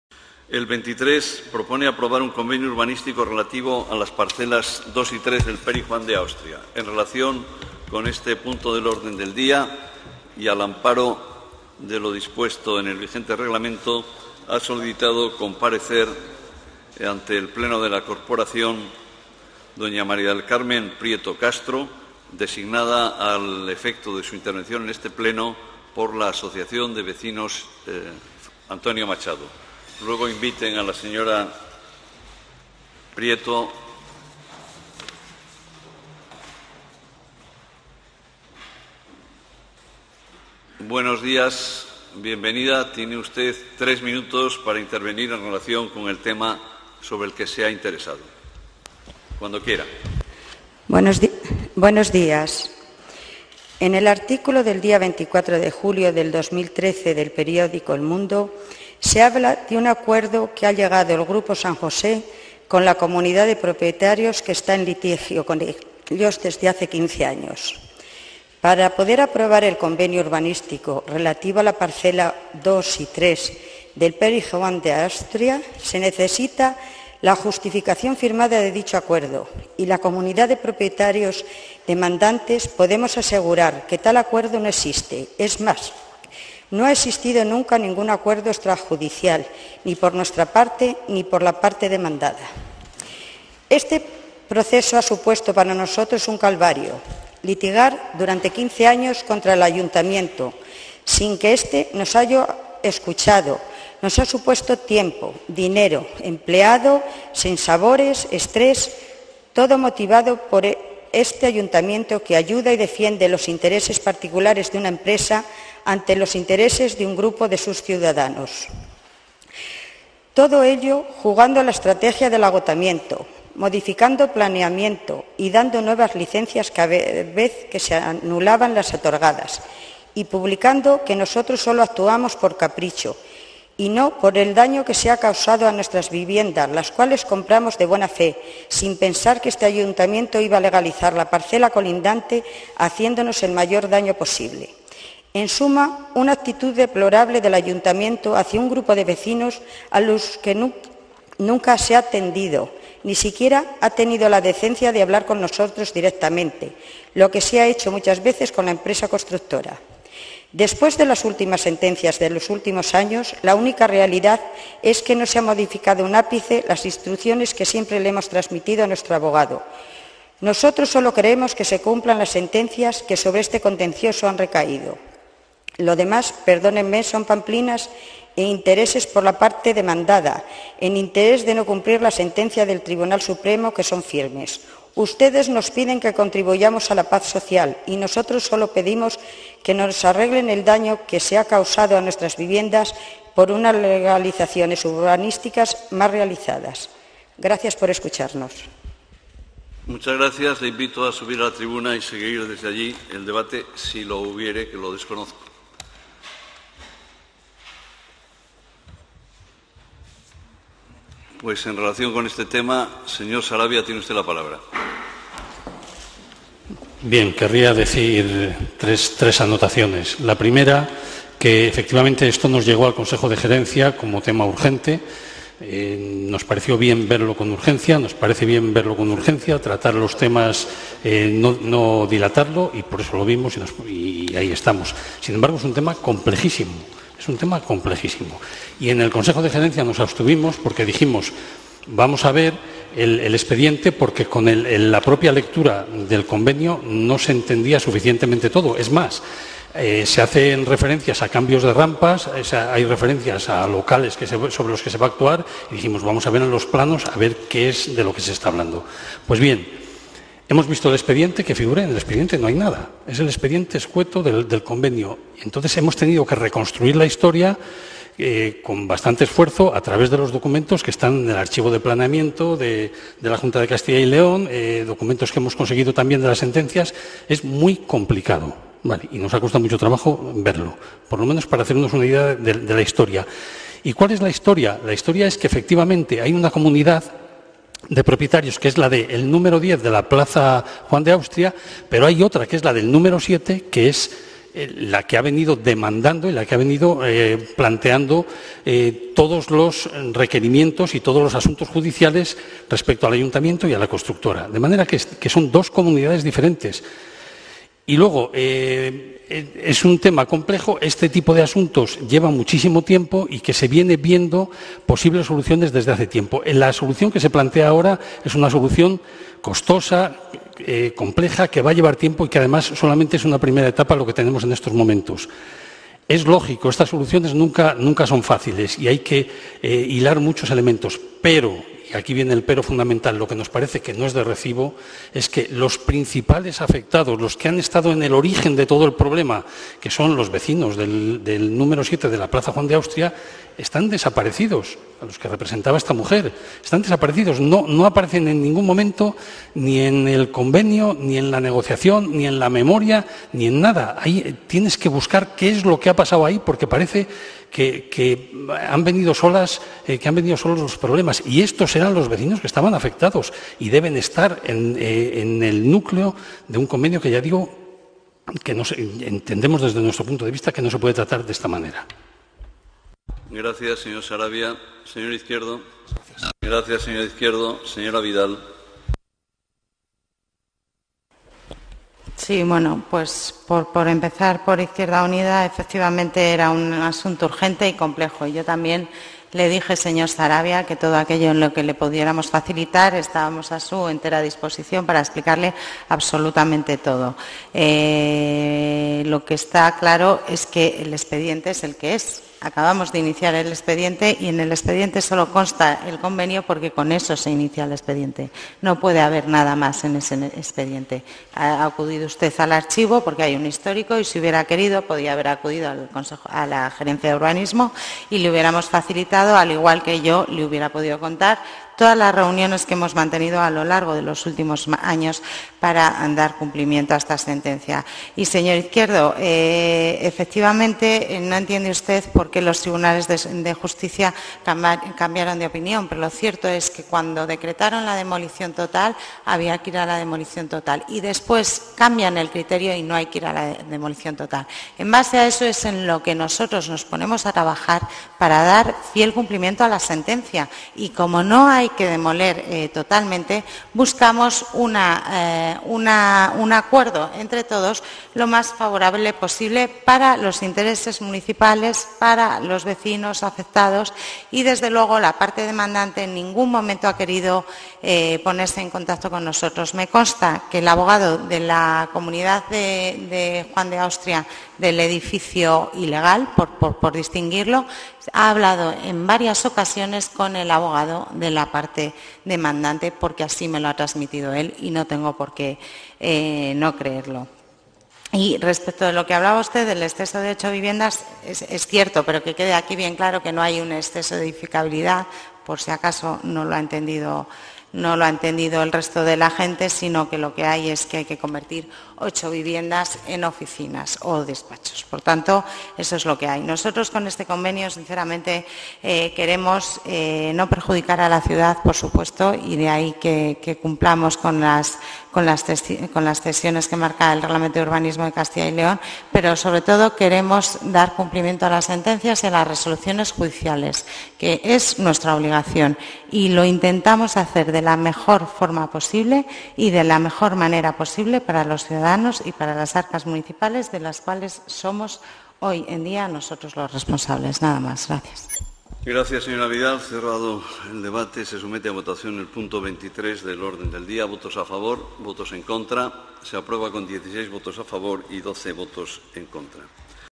Manuel Saravia: sobre el pisoteo del ayuntamiento a los vecinos de la Plaza Juan de Austria
Criticamos como el Ayuntamiento defiende al Grupo San Jose en vez de a los vecinos de los números 7 y 10 de la Plaza Juan de Austria, legalizando parcelas reiteradamente en contra de las sentencias judiciales.